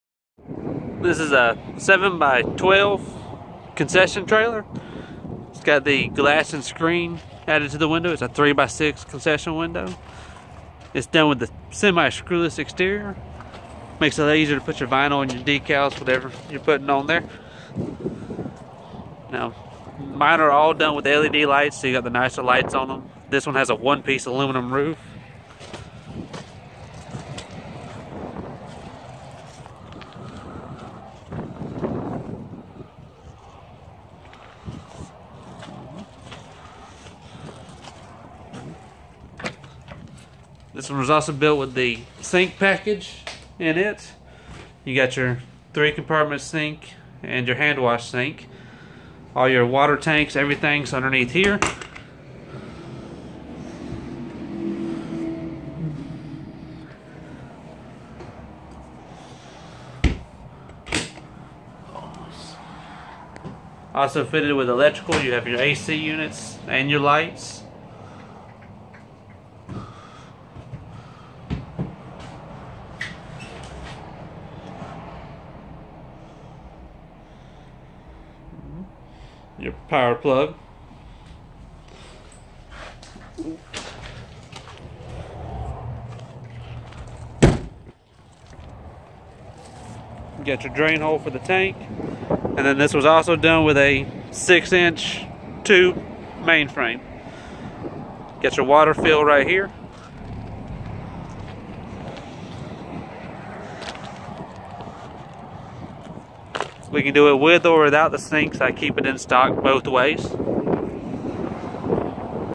7 x12 concession trailer walk around
7 x12 concession trailer with sinks. Finished interior. Electoral package. AC with head. 3 x6 window with glass and screen. Walk around review. Vending trailer new.

best trailers
macon ga